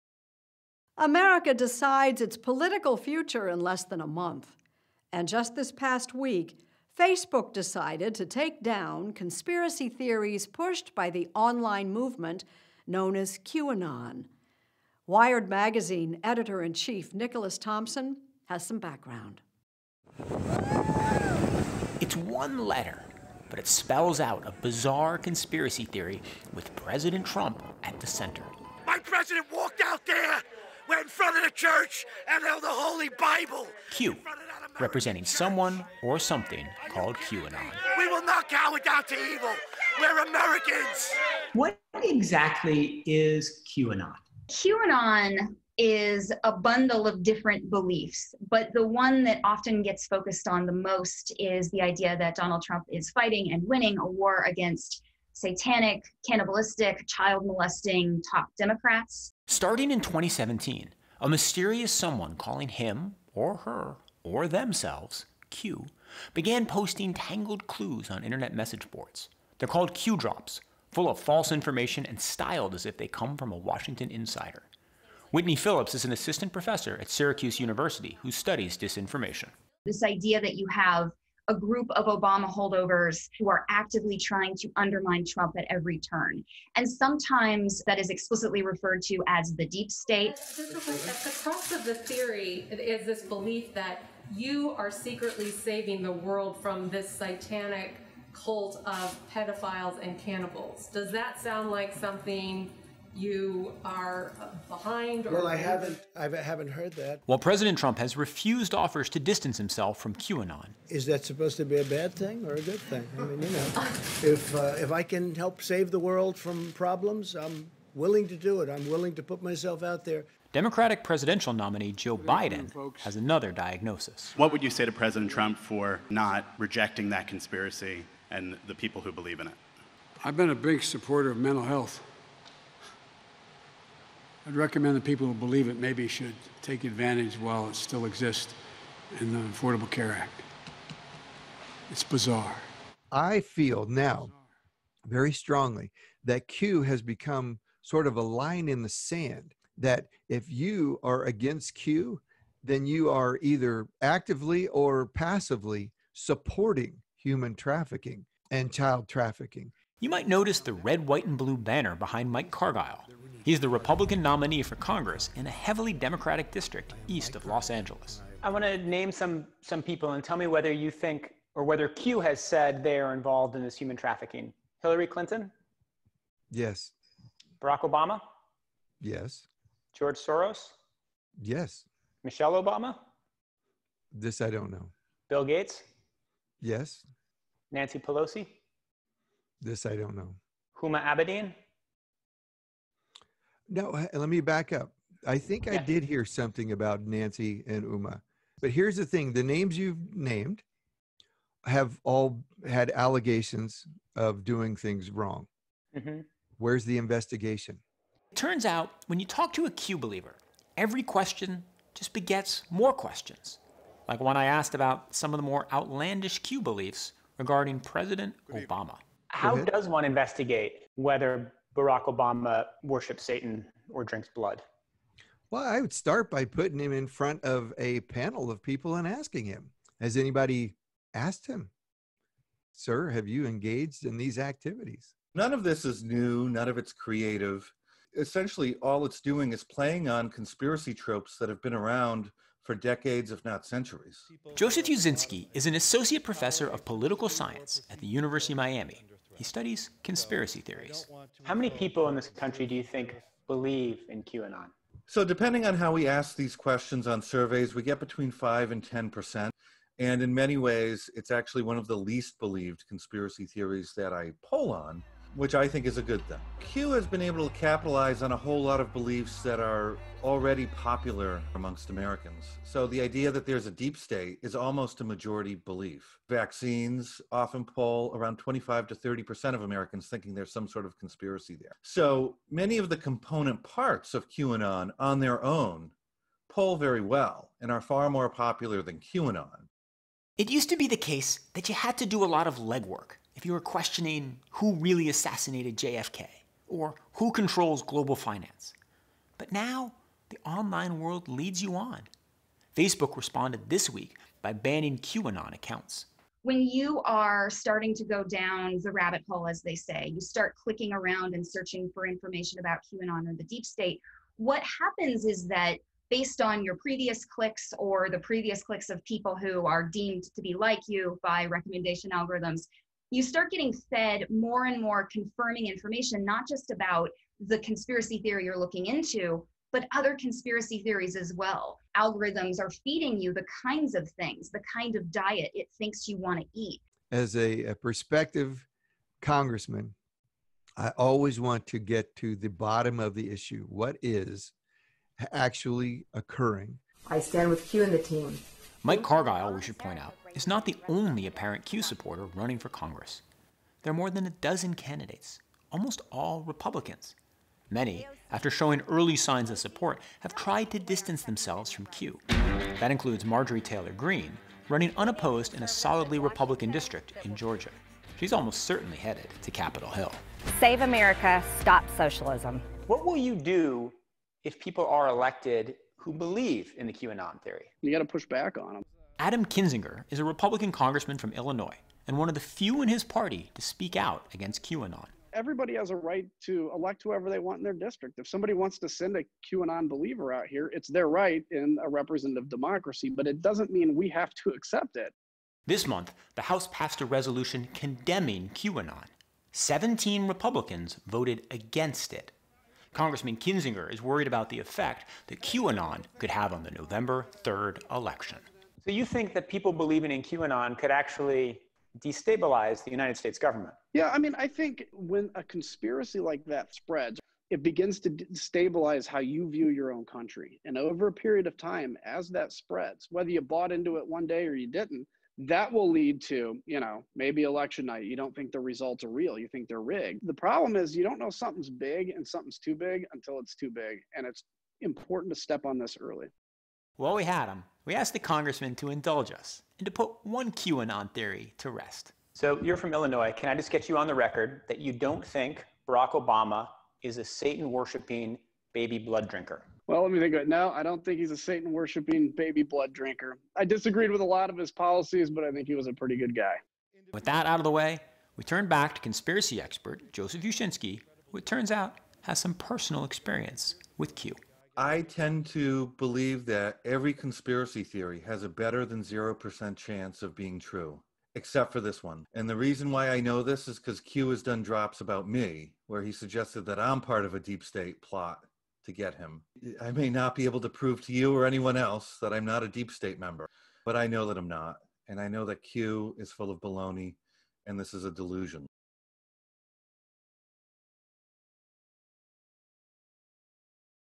QAnon and the spread of conspiracy theories
Followers of the online conspiracy theorist "QAnon" have propagated outlandish disinformation about secret plots by a "deep state" cabal of Satanic, cannibalistic child-traffickers out to "get" President Trump. How do these anonymous accusations spread, and what happens if Q believers are elected to office? Wired magazine editor-in-chief Nicholas Thompson reports.